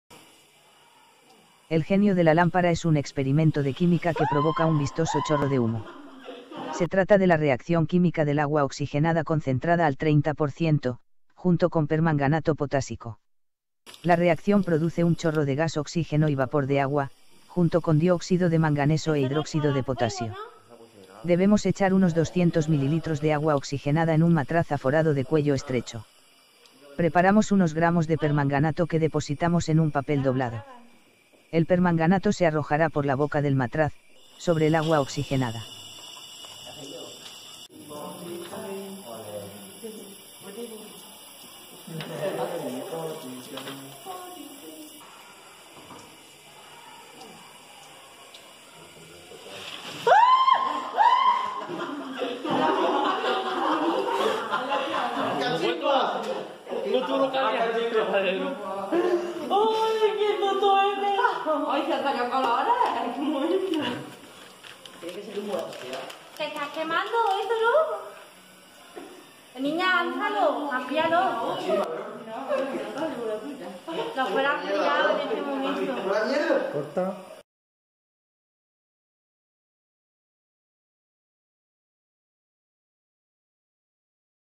Experimento el genio de la lampara: reaccion agua oxigenada concentrada con permanganato potasico.
El genio de la lámpara es un experimento de química que provoca un vistoso chorro de humo. Se trata de la reacción química del agua oxigenada concentrada al 30%, junto con permanganato potásico.
La reacción produce un chorro de gas oxígeno y vapor de agua, junto con dióxido de manganeso e hidróxido de potasio.

Debemos echar unos 200 mililitros de agua oxigenada en un matraz aforado de cuello estrecho. Preparamos unos gramos de permanganato que depositamos en un papel doblado. El permanganato se arrojará por la boca del matraz, sobre el agua oxigenada.